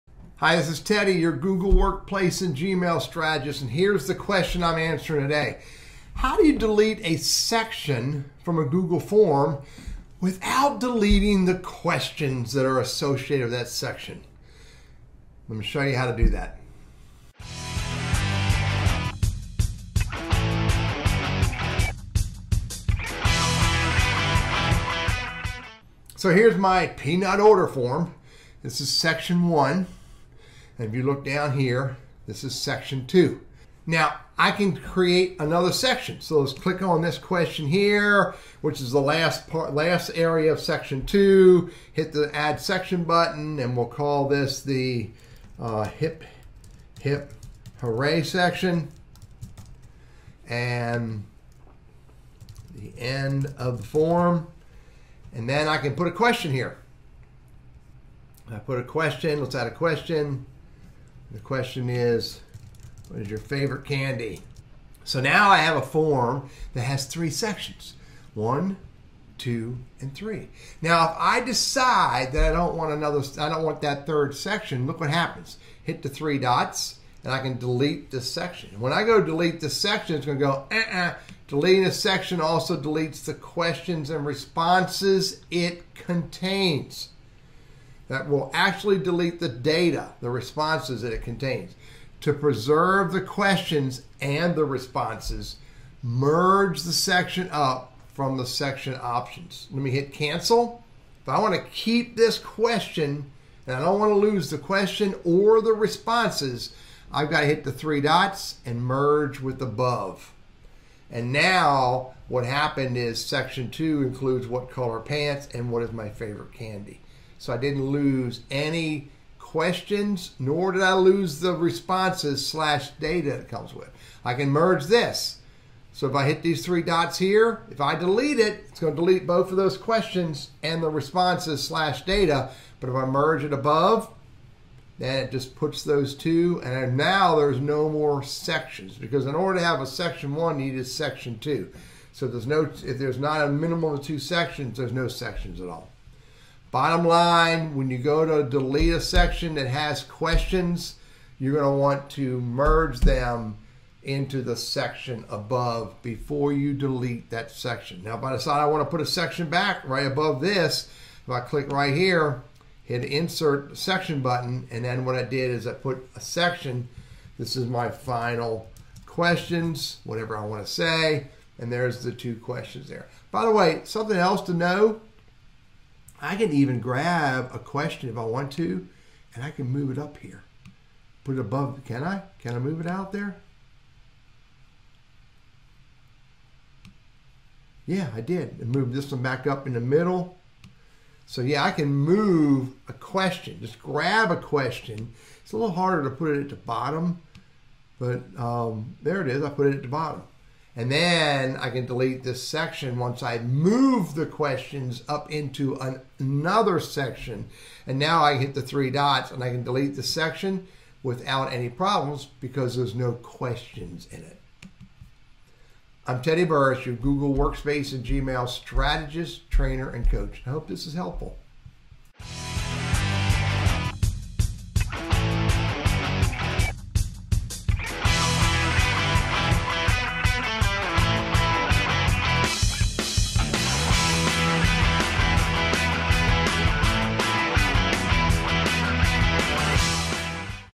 How can I delete a Google Form Section without losing the Form questions or Responses?
You have to either merge the section up into the section above or manually move the questions to another section before you delete a section.